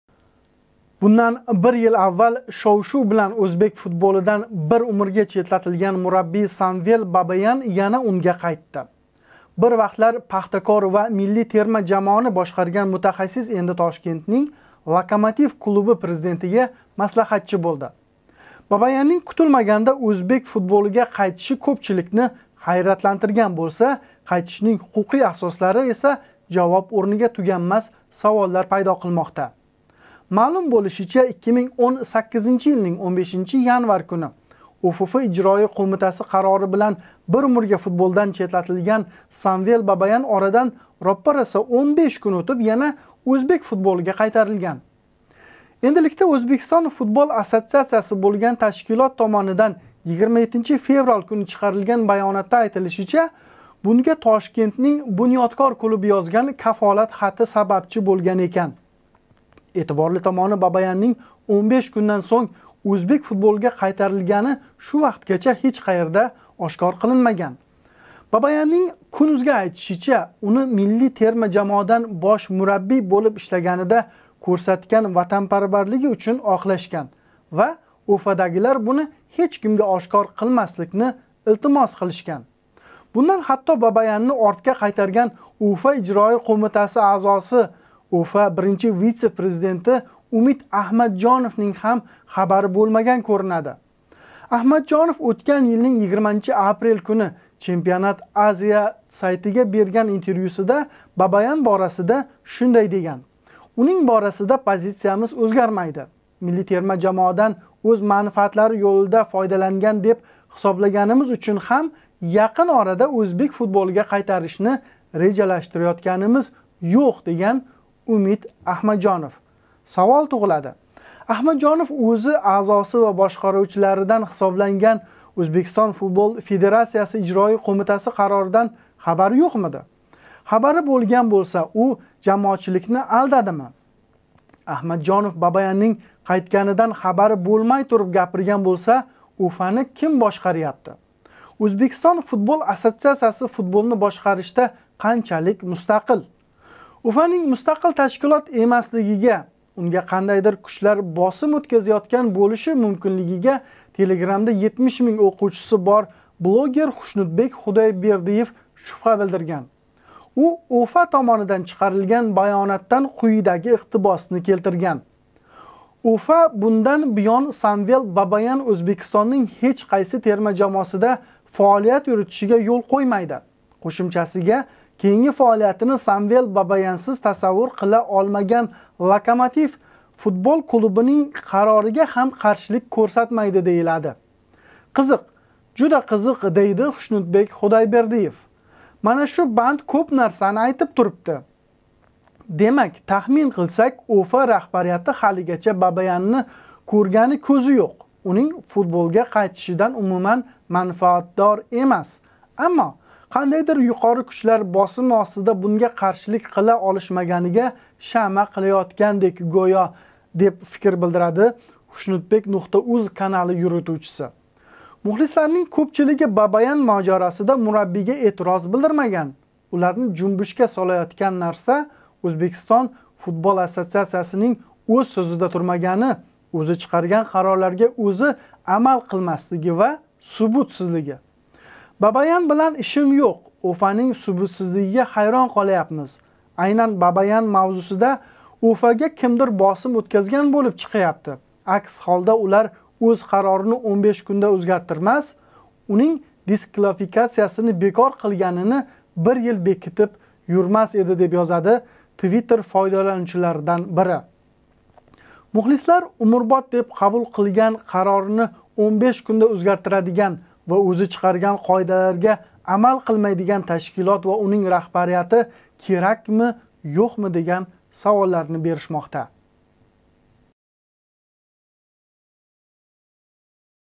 Бабаян машмашаси: Умрбод Ўзбек футболидан четлатилган мураббий қандай қайтди?
Бундан бир йил аввал шов-шув билан ўзбек футболидан бир умрга четлатилган мураббий Самвел Бабаяннинг яна унга қайтиши жавоблардан кўп саволларни пайдо қилди. Мухлислар Бабаяннинг қайтишидан эмас, балки Ўзбекистон футбол ассоциациясининг "субутсизлиги" ва нопрофессионаллигидан норози бўлмоқда.

Бир вақтлар Пахтакор ва Миллий терма жамоани бошқарган мутахассис энди Тошкентнинг "Локомотив" клуби президентига маслаҳатчи бўлди.

Бабаяннинг кутилмаганда ўзбек футболига қайтиши кўпчиликни ҳайратлантирган бўлса, қайтишнинг ҳуқуқий асослари эса жавоб ўрнига туганмас саволлар пайдо қилмоқда.

Маълум бўлишича, 2018 йилнинг 15 январ куни ЎФФ Ижроия Қўмитаси қарори билан бир умрга футболдан четлатилган Самвел Бабаян орадан роппа роса 15 кун ўтиб яна ўзбек футболига қайтарилган.

Эндиликда Ўзбекистон Футбол ассоциацияси бўлган ташкилот томонидан 27 феврал куни чиқарилган баёнотда айтилишича, бунга Тошкентнинг "Бунёдкор" клуби ёзган кафолат хати сабабчи бўлган экан.

Эътиборли томони Бабаяннинг 15 кундан сўнг ўзбек футболига қайтарилгани шу вақтгача ҳеч қаерда ошкор қилинмаган.

Бабаяннинг кун.узга айтишича, уни миллий терма жамоада бош мураббий бўлиб ишлаганида кўрсатган ватанпарварлиги учун оқлашган. Ва ЎФАдагилар буни ҳеч кимга ошкор қилмасликни илтимос қилишган.

Бундан ҳатто Бабаянни ортга қайтарган ЎФА Ижроия Қўмитаси аъзоси, ЎФА 1- вице президенти Умид Аҳмаджоновнинг ҳам хабари бўлмаган кўринади.

Аҳмаджонов ўтган йилнинг 20 апрел куни championat.asia сайтига берган интервюсида Бабаян борасида шундай деган.

"Унинг борасида позициямиз ўзгармайди. Миллий терма жамоадан ўз манфаати йўлида фойдаланган деб ҳисоблаганимиз учун ҳам яқин орада ўзбек футболига қайтаришни режалаштираётганимиз йўқ", деган Умид Аҳмаджонов.

Савол туғилади, Аҳмаджонов ўзи аъзоси ва бошқарувчиларидан ҳисобланган ЎФФ ижроия қўмитаси қароридан хабари йўқмиди? Хабари бўлган бўлса у жамоатчиликни алдадими? Аҳмаджонов Бабаяннинг қайтганидан хабари бўлмай туриб гапирган бўлса ЎФАни  ким бошқаряпти? Ўзбекистон Футбол ассоциацияси футболни бошқаришда қанчалик мустақил?

ЎФАнинг мустақил ташкилот эмаслигига, унга қандайдир кучлар босим ўтказаётган бўлиши мумкинлигига Телеграмда 70 минг ўқувчиси бор блогер Хушнудбек Худайбердиев шубҳа билдирган.

У ЎФА томонидан чиқарилган баёнотдан қуйидаги иқтибосни келтирган.

"ЎФА бундан буён Самвел Бабаян Ўзбекистоннинг ҳеч қайси терма жамоасида фаолият юритишига йўл қўймайди. Қўшимчасига, кейинги фаолиятини Самвел Бабаянсиз тасаввур қила олмаган "Локомотив" футбол клубининг қарорига ҳам қаршилик кўрсатмайди", дейилади.

Қизиқ, жуда қизиқ., дейди Хушнудбек Худайбердиев.

"Мана шу банд кўп нарсани айтиб турибди. Демак, тахмин қилсак, ЎФА раҳбарияти ҳалигача Бабаянни кўргани кўзи йўқ, унинг футболга қайтишидан умуман манфаатдормас. Аммо қандайдир «юқори кучлар» босими остида бунга қаршилик қила олишмаганига шаъма қилаётгандек гўё", дея фикр билдиради xushnudbek.uz канали юритувчиси.

Мухлисларнинг кўпчилиги Бабаян можаросида мураббийга эътироз билдирмаган. Уларни жунбушга солаётган нарса Ўзбекистон Футбол ассоциациясининг ўз сўзида турмагани, ўзи чиқарган қарорларга ўзи амал қилмаслиги ва субутсизлиги.

"Бабаян билан ишим йўқ, ЎФАнинг субутсизлигига ҳайрон қолаяпмиз. Айнан Бабаян мавзусида ЎФАга кимдир босим ўтказган бўлиб чиқяпти. Акс ҳолда улар ўз қарорини 15 кунда ўзгартирмас, уни дисфликациясини бекор қилганини 1 йил бекитиб юрмас эди", деб ёзади твиттер фойдаланувчиларидан бири.

Мухлислар умрбод деб қабул қилган қарорини 15 кунда ўзгартирадиган ва ўзи чиқарган қоидаларга амал қилмайдиган ташкилот ва унинг раҳбарияти керакми йўқми деган саволларни беришмоқда.